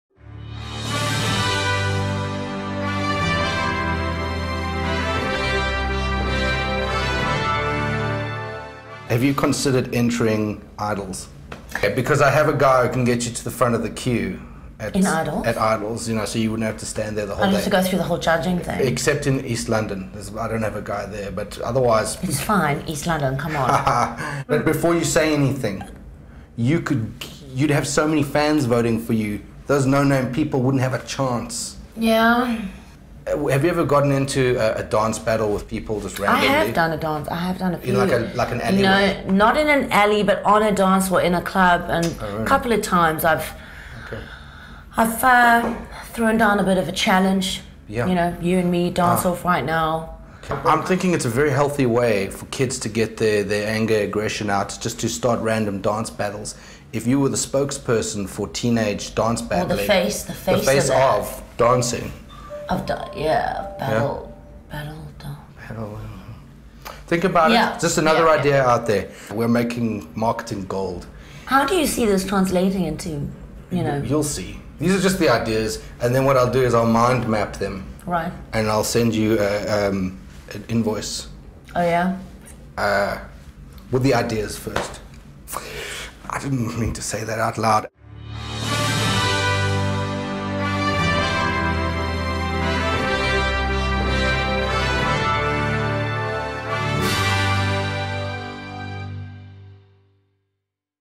Tamara Dey & David Leno, How To Get Far In Idols If You're Already Famous Part 3
Tamara Dey and David Leno talk about her own perfume, Flash versus Mac Republic and her winning idols as an already established star.